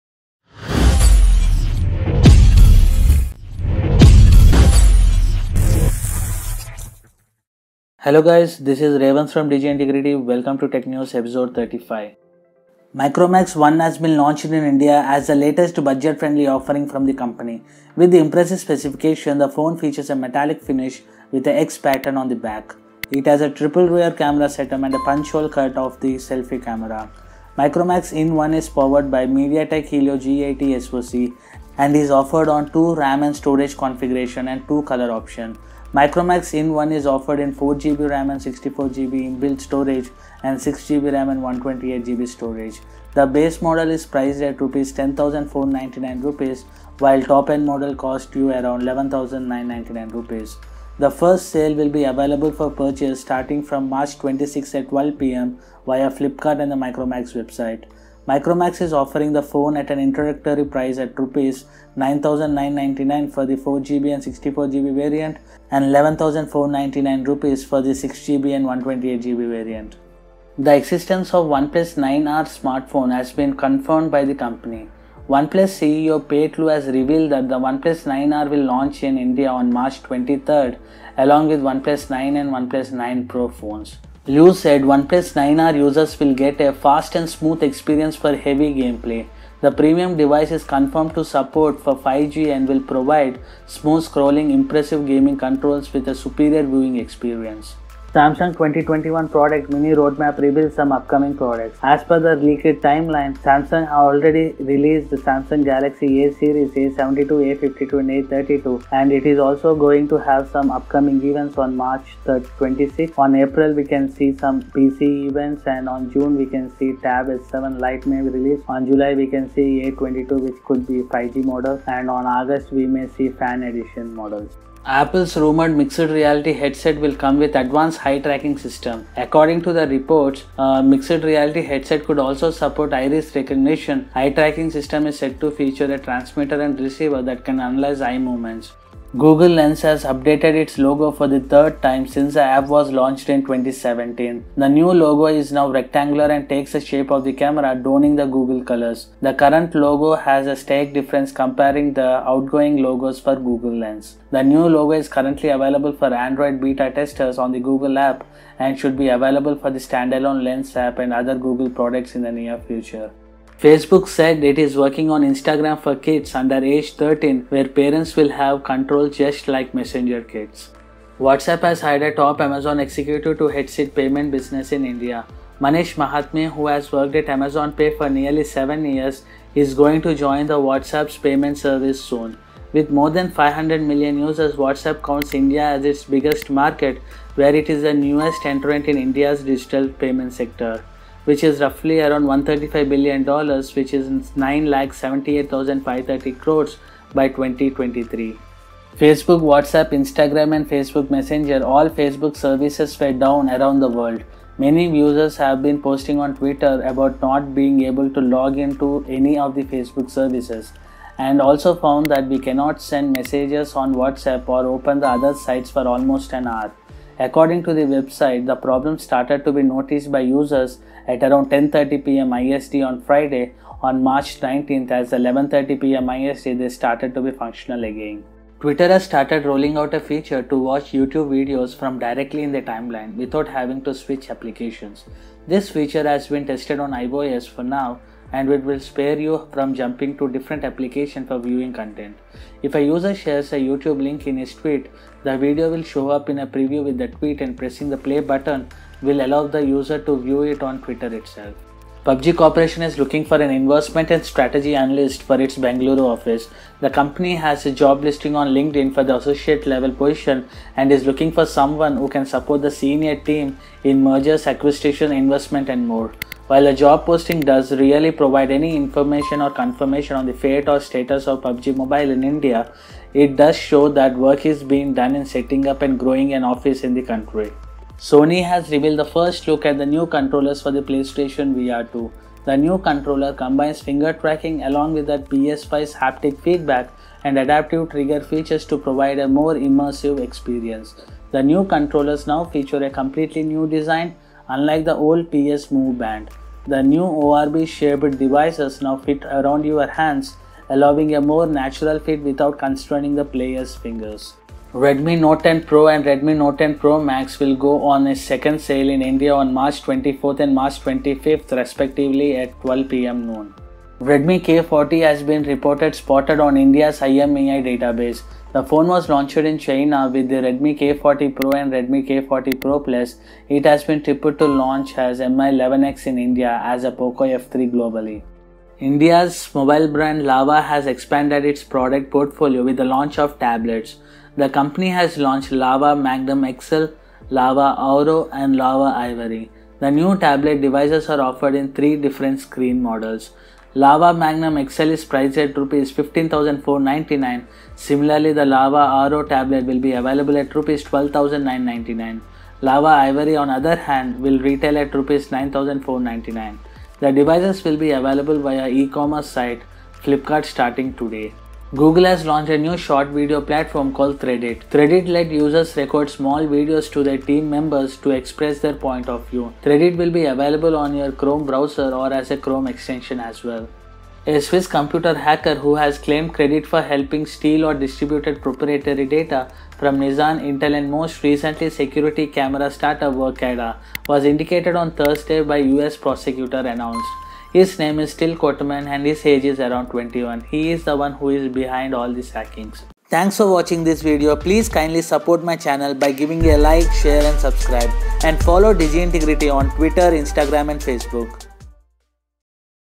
TechNews 35 | Micromax In 1, Pubg, Oneplus 9R, Samsung Timeline, Whatsapp Server Down, Threadit Etc
Video Highlights
Introduction-(0:00​​​​​​​​​​)
Micromax In 1 with MediaTek Helio G80 SoC, 5,000mAh battery launched in India-(0:12)
OnePlus 9R 5G launch confirmed, will come with affordable price in India-(1:10)
Timeline For Samsung Foldables & Galaxy S21 FE Revealed-(1:38)
Apple Mixed Reality Headset Tipped to Come With Advanced Eye Tracking, Iris Recognition-(2:05)
Google Lens Logo Gets a Complete Redesign to Emphasise Its Camera Function-(2:19)
Instagram for Kids Under The Age of 13 Reportedly Under Development, Internal Communication Suggests-(2:05)
WhatsApp Said to Hire Amazon Pay’s Manesh Mahatme to Lead India Payments Business-(2:51)
Facebook, WhatsApp, Instagram Were Briefly Down for Users Around the World-(3:19)
Twitter Starts Letting iOS Users View YouTube Videos In-App-(3:48​​​​​​​)
PUBG Corporation Is Hiring in India, Looking for Investment and Strategy Analyst-(4:12)
Sony reveals all-new PlayStation VR controller for the PS5-(4:38)
Redmi Note 10 Pro Next Sale on March 24, Redmi Note 10 Pro Max on March 25-(5:04)
Redmi K40 Alleged IMEI Listing Hints at Imminent India Launch, Could Arrive as Mi 11X-(5:14​)
Lava Magnum XL, Lava Aura, and Lava Ivory tablets launched, price starts at Rs 9,499-(5:28)
Google launches Threadit, a TikTok-like short video platform for professionals-(6:02​)
Swiss hacker indicted after claiming credit for breaching Nissan, Intel-(6:14​)
Thanks for watching-(6:32​)